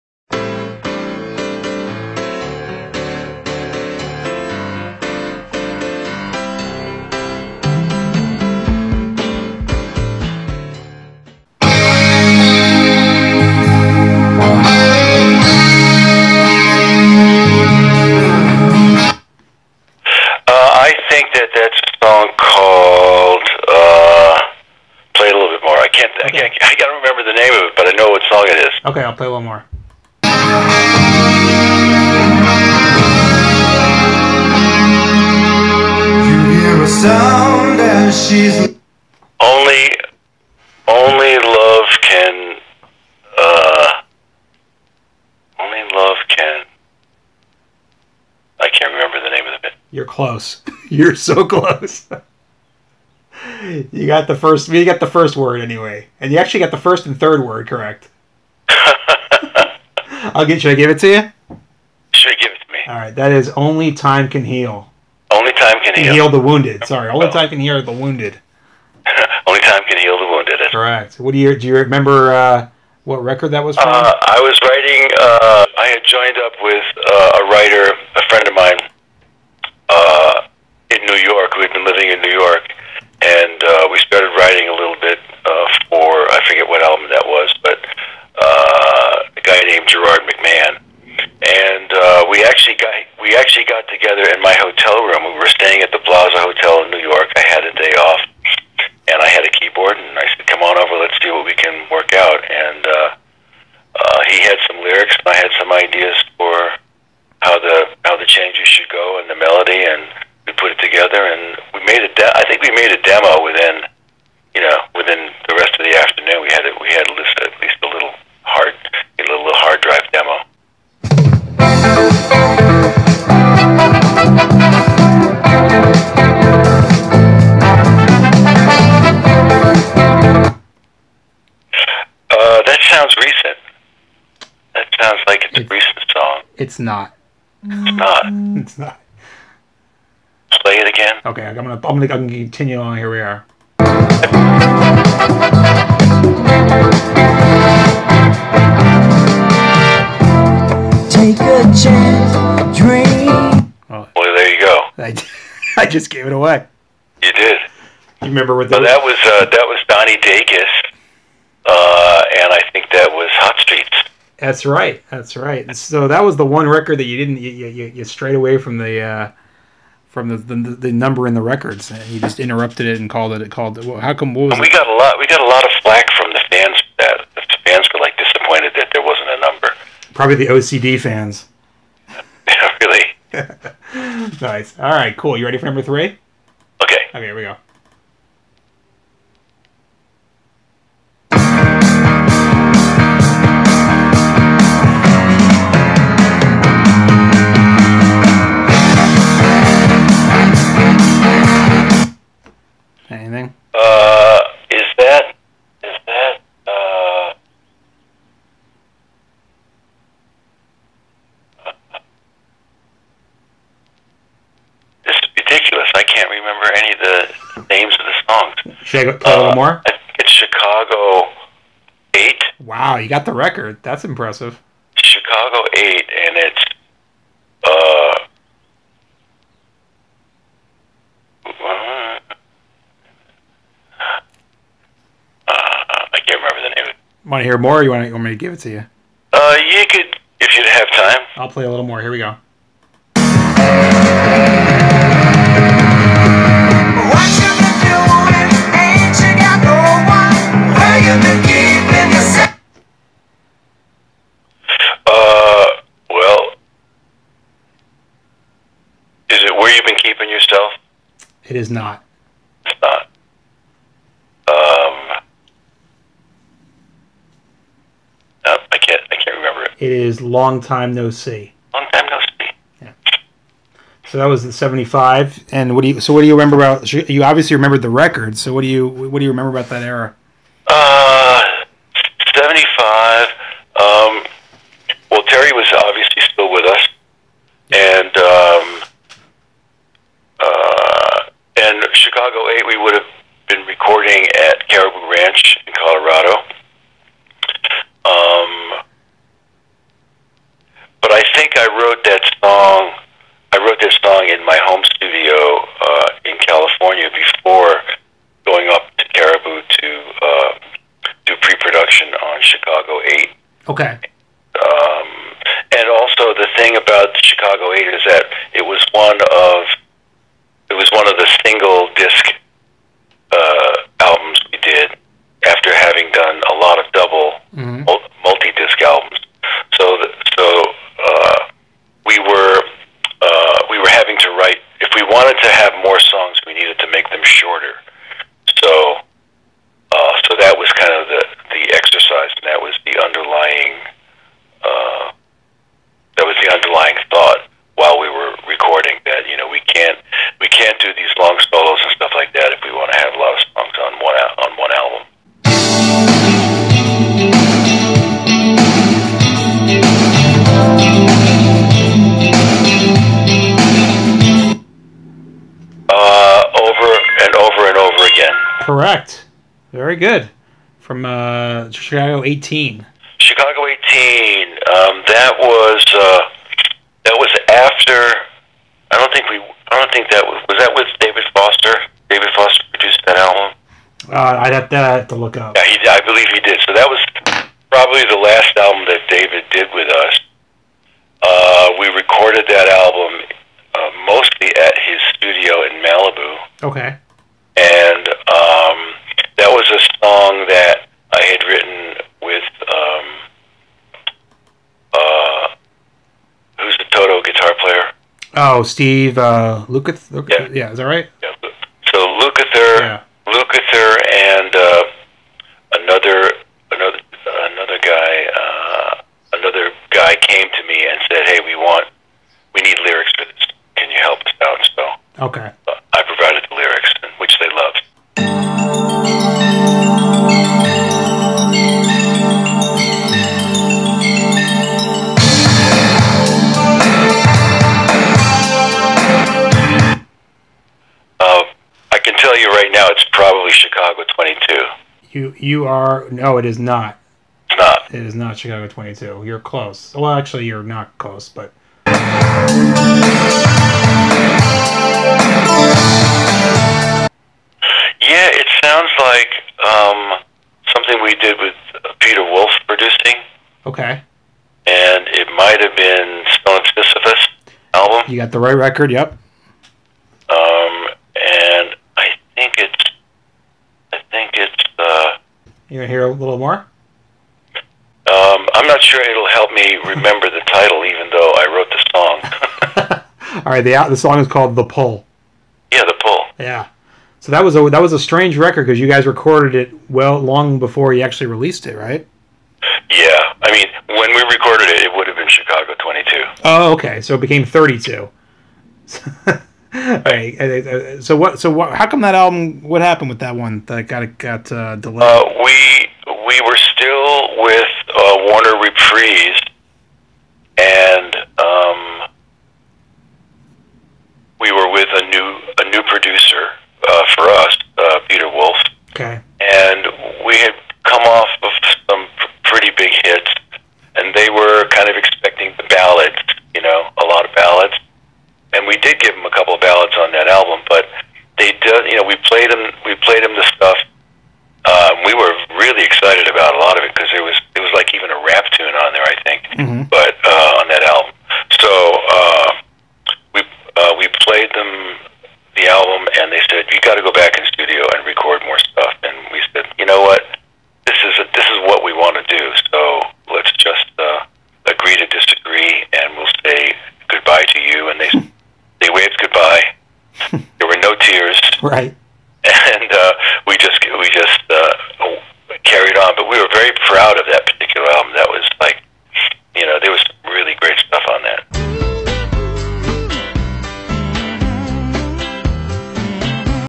Robert Lamm of Chicago Plays Rockers' Block on BlastEcho
Robert Lamm of Chicago plays the "Name That Tune"-like game (only with his OWN songs) Rockers' Block exclusively on BlastEcho